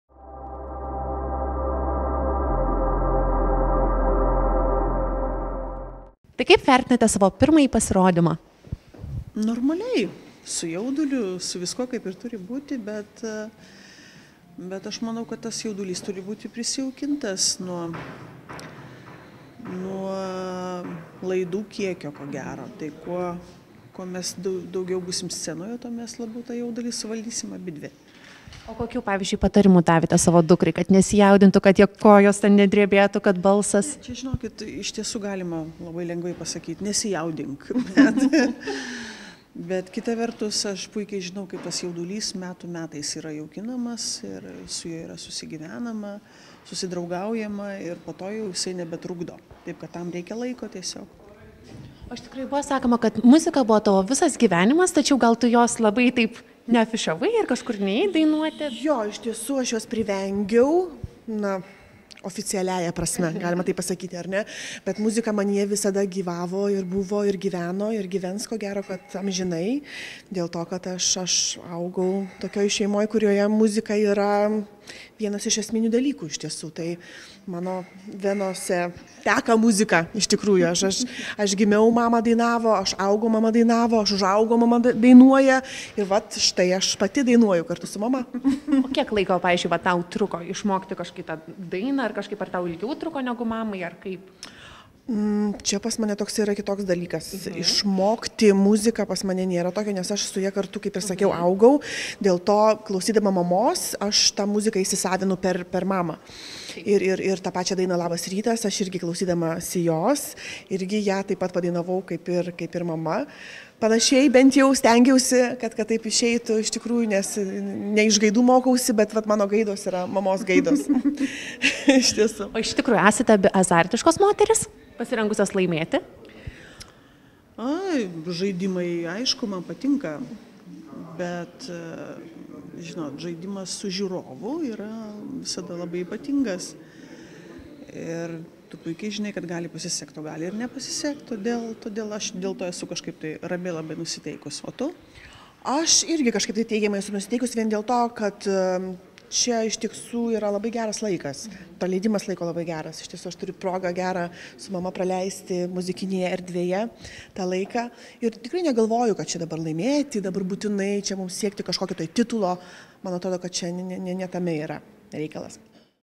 Rosita Čivilytė su dukra Agneta
Rosita Čivilytė - scenos senbuvė, prie kurios šiais metais projekto „Žvaigždžių duetų" scenoje prisidėjo jos dukra Agneta Gabalytė. Iš pirmo žvilgsnio net negalėtumei pasakyti, kad tai - mamos ir dukros duetas, nes abi moterys atrodo kaip sesės.

Tačiau tarpusavio ryšis tarp Rositos ir Agnetos yra nenuginčijamas. Mama dukrai nuolatos duoda įvairių patarimų, mat per tiek metų yra sukaupta nemažai patirties.

Skaitykite daugiau: Rosita Čivilytė apie jaudulį: jis jaukinamas metų metus - Naujienų portalas | Alfa.lt